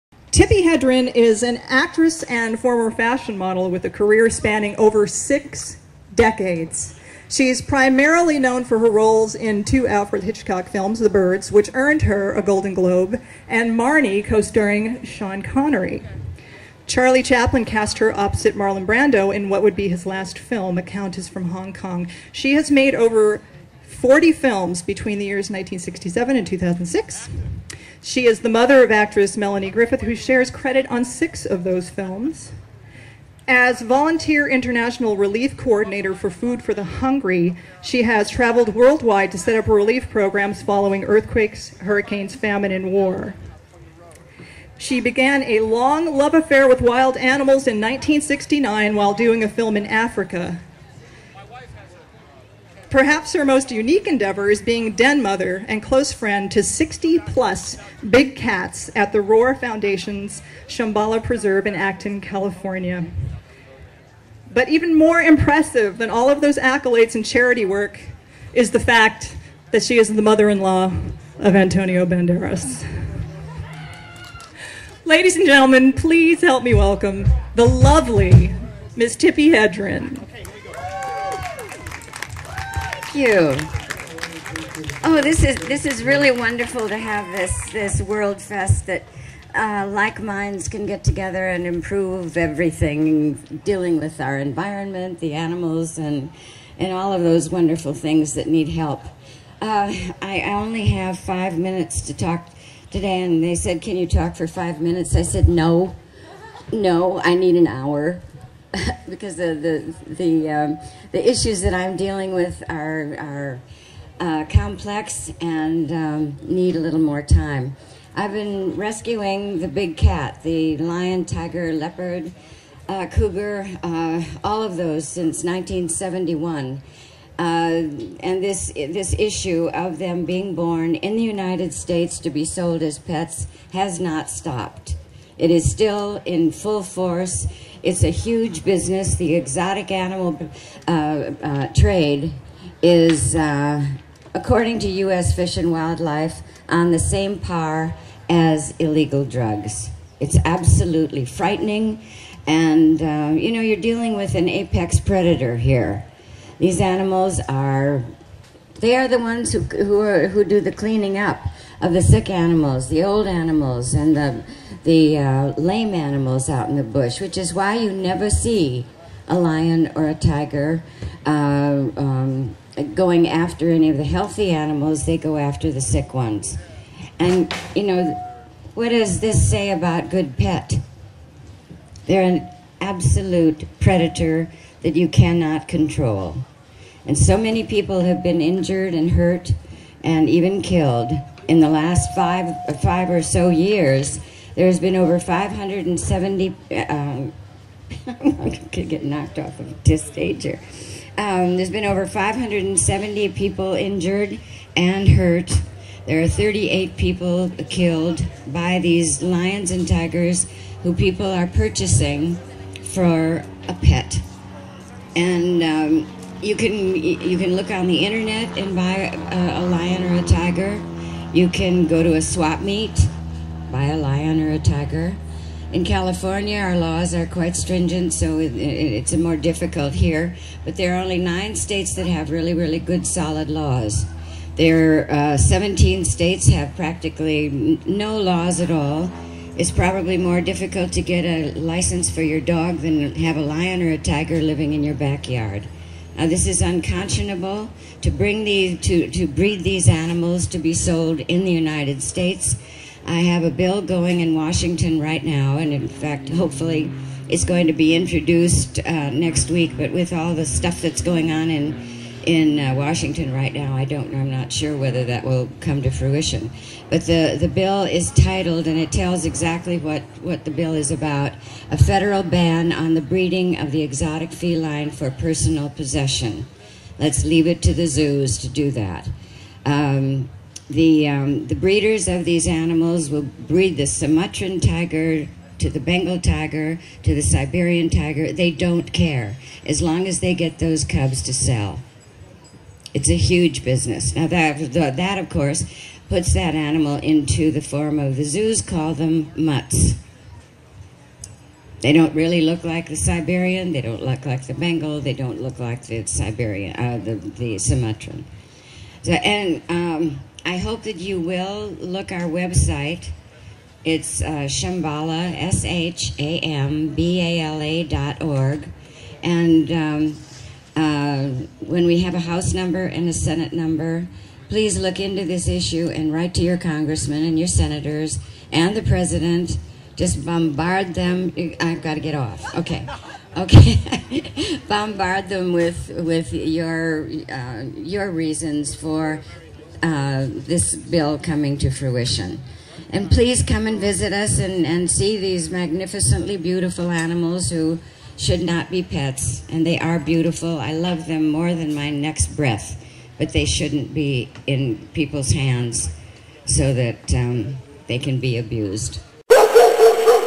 Tippi Hedren of the Roar Foundation and the Shambala Preserve introduced by Mariana Tosca
The Roar Foundation, founded in 1983 by Tippi Hedren, exists solely to support the Shambala Preserve.  Their mission is to educate the public about the dangers of private ownership of exotic animals.  Huge numbers of dangerous animals are bred and sold in the United States for illegal purposes.  Private ownership presents a grave danger to the public and is cruel and unfair.  More stringent legislation is needed to prohibit breeding and selling.  They are actively involved on federal as well as state levels.

Shambala is home to more than 70 big cats: lions, tigers, cougars, black and spotted leopards, servals, bobcats, Asian leopard cats, a jungle cat, a lynx, and one very magnificent liger.  All have come to the Preserve after confiscation by authorities such as California Fish and Game, the United States Department of Agriculture, the ASCPA, and various Humane Societies.  They are from roadside zoos and private citizens who realize that they have purchased an animal that they can no longer handle.  The exotic cat trade is a huge business, just under illegal drugs, according to U.S. Fish and Wildlife.

Once an animal is brought to Shambala, it remains there for the rest of its life.  As a true sanctuary, they do not buy, breed, sell, trade, or subject our animals to commercial use.  Their only purpose is to allow these magnificent animals to live out their lives with love and dignity.  Each "Wild One" has the best human, nutritional, medical, emotional, and mental care possible.

Produced by Jeff Fleiss, Founder of AnimalNews.info, Spay Neuter USA and DoggyTV